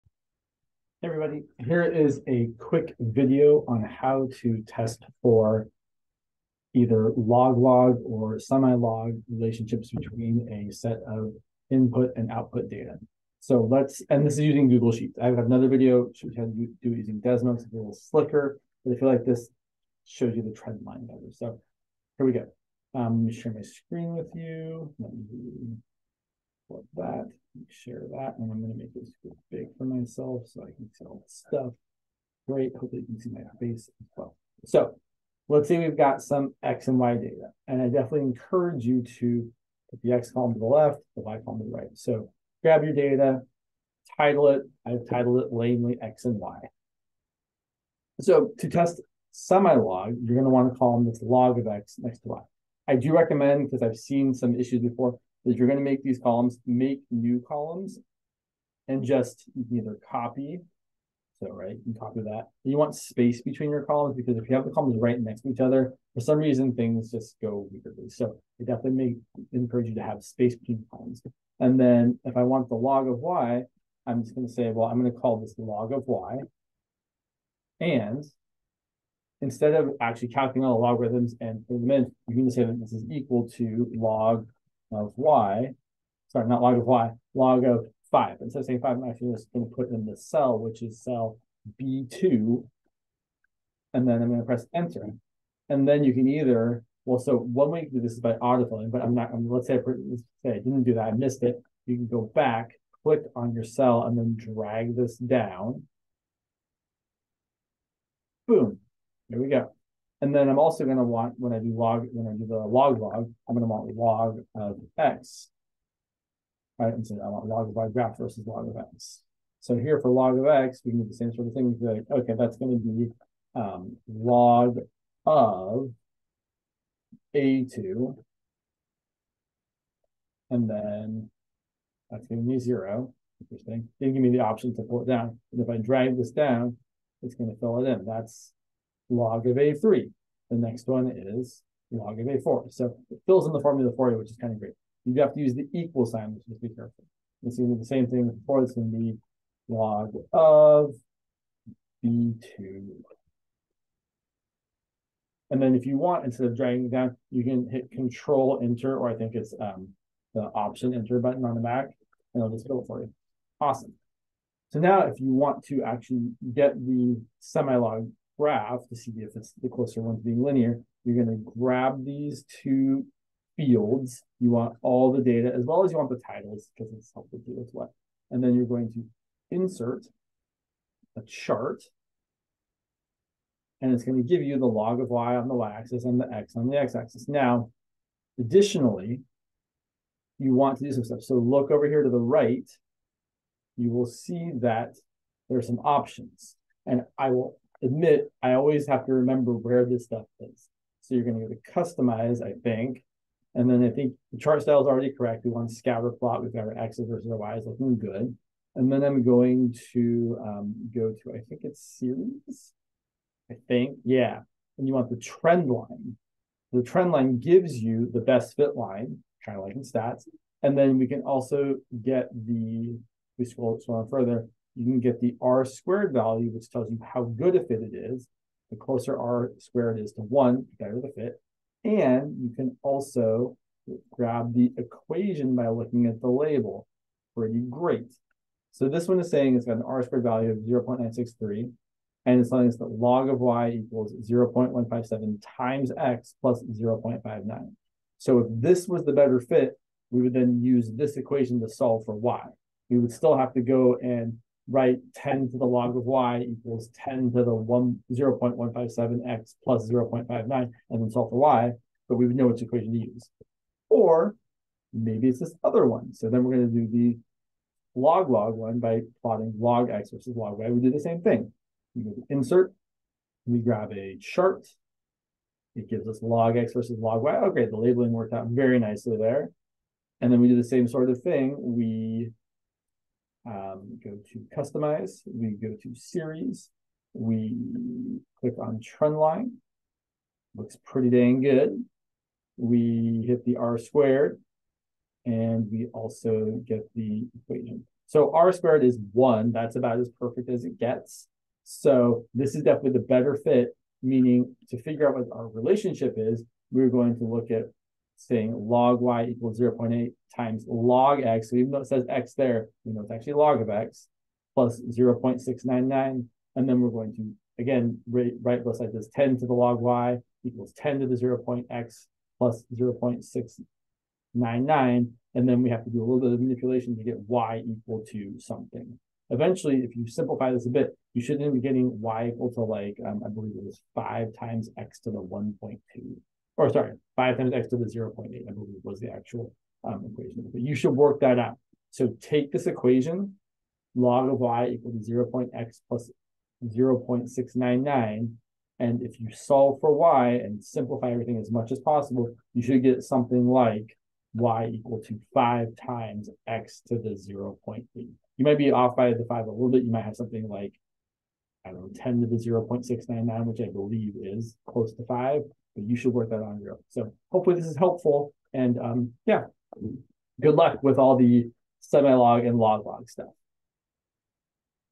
How to Determine Semi-Log vs. Log-Log in Google Sheets
Note!
At 5:20 I said to solve for y we would need to do "10 to the log(y) equals 10 to the 0.8 x plus 0.699" but

What I should have said actually is "10 to the log(y) equals 10 to the 0.8 times log(x) plus 0.699" since that was a log-log (and not semi-log) relationship.